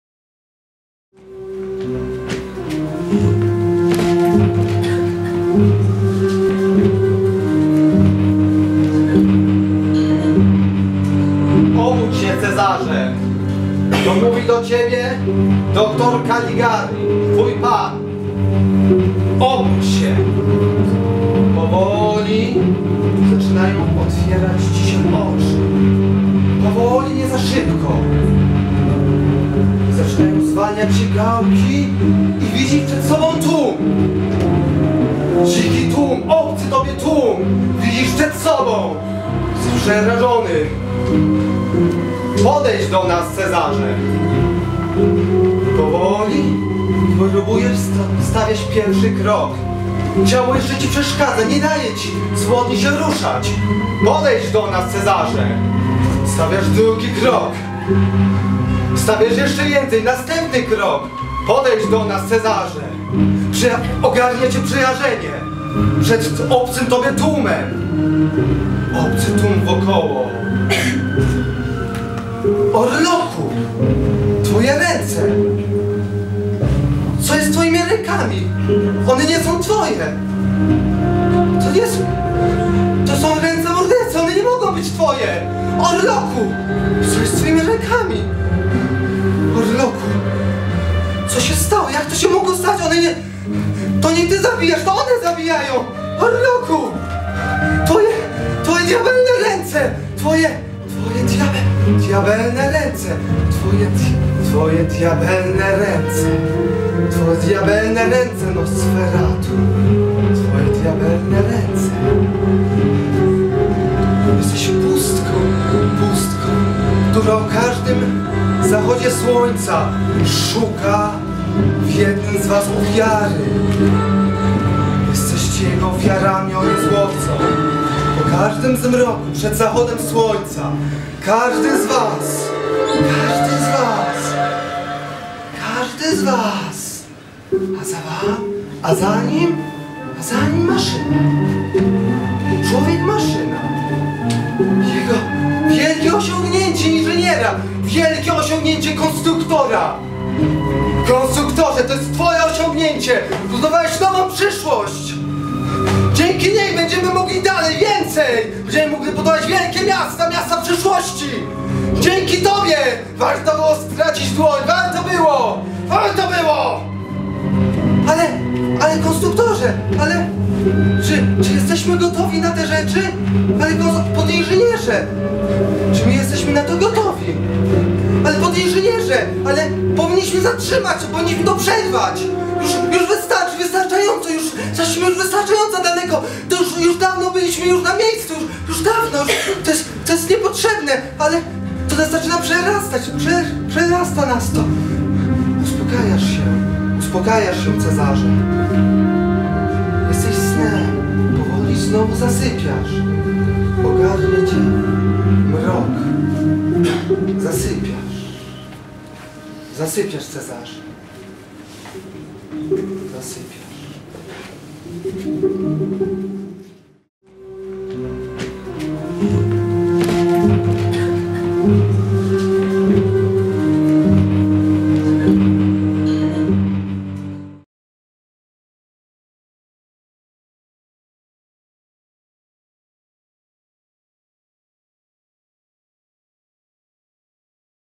TodMachine | Etiuda - Gabinet zapomnianych bohaterów
Projekt filmowy TodMachine - nowe spojrzenie na zapomniane kino!

Był to występ mający przypomnieć kilku najważniejszych bohaterów niemieckiego ekspresjonizmu. W roli hipnotyzera wcielił się Bogusław Kornaś (reżyser filmu TodMachine), bohaterów filmowych kreował Grzegorz Stokłosa (odtwórca głównej roli w filmie TodMachine). Występ zaprezentowany był w ramach Tarnowskiej Szopki Literackiej w Klubie Przepraszam w 2013 roku.

Pomoc Techniczna - Marcin Halski
Makijaż - Izabella Wielgus